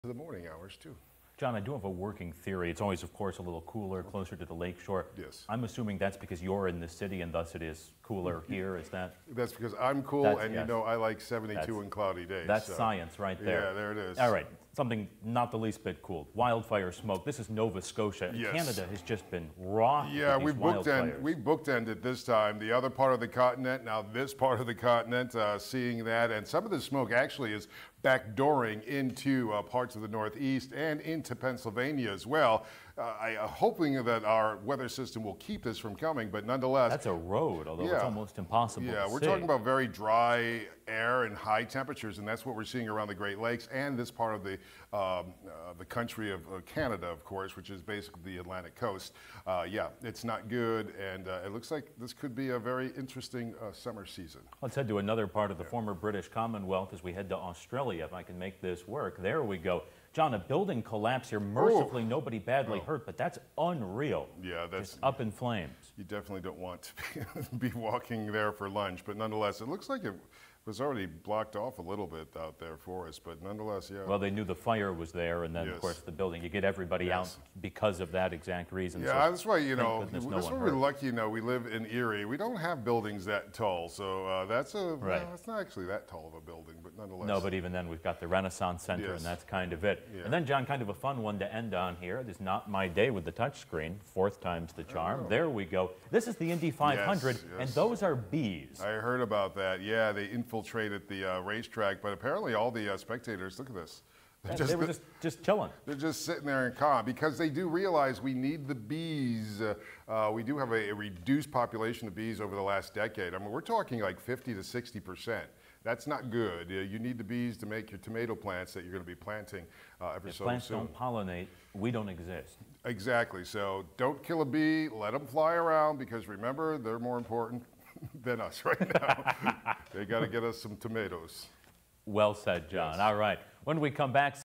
Trending Weather: Canada Wildfire, Australia Building Collapse, Indy 500
Residents in upper Tantallon were being evacuated on Sunday, May 28, as a wildfire there continued to spread quickly.
Building collapse in Sydney, Australia. No one was badly injured.